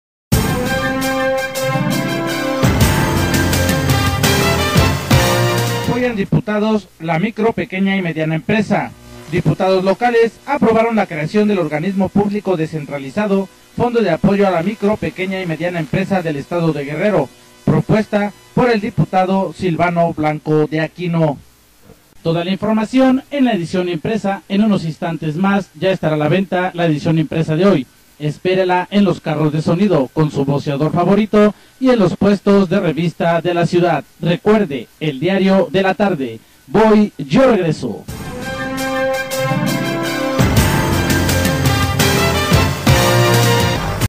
APOYAN DIPUTADOS LA MICRO MEDIANA Y PEQUEÑA EMPRESA
Diputados locales aprobaron la creación del Organismo Público Descentralizado Fondo de Apoyo a la Micro, Pequeña y Mediana Empresa del Estado de Guerrero, propuesta por el diputado Silvano Blanco Deaquino.